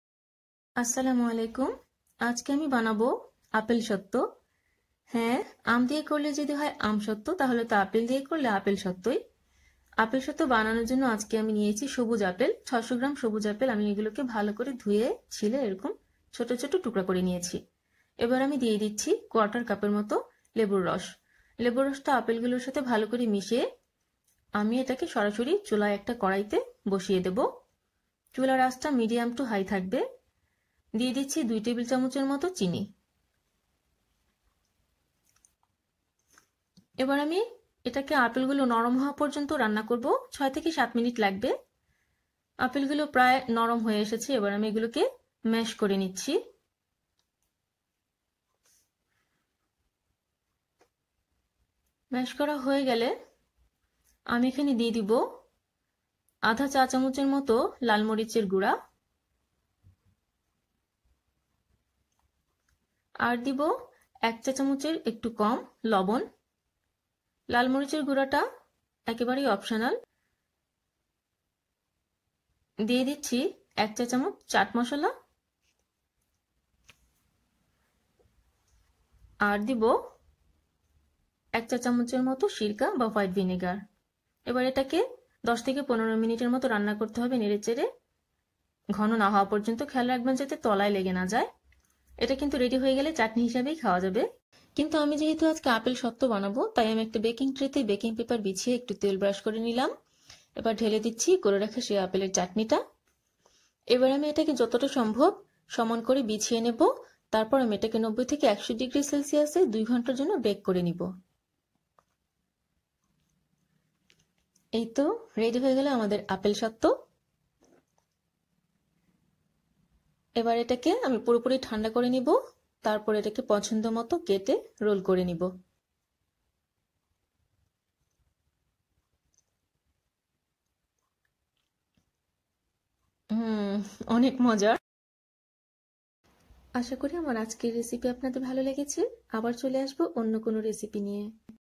🍏Apple papad💚😋 with Green Apples ( Granny Smith)
mouth watering Apple papad 😋

Ingredients:
600 gm Green Apples  (Granny Smith)
1/4 cup fresh lemon juice
2 tbsp sugar
1/2 tsp red chilli powder  (optional)
less than 1 tsp salt (or to taste)
1 tsp chat masala
1 tsp white vinegar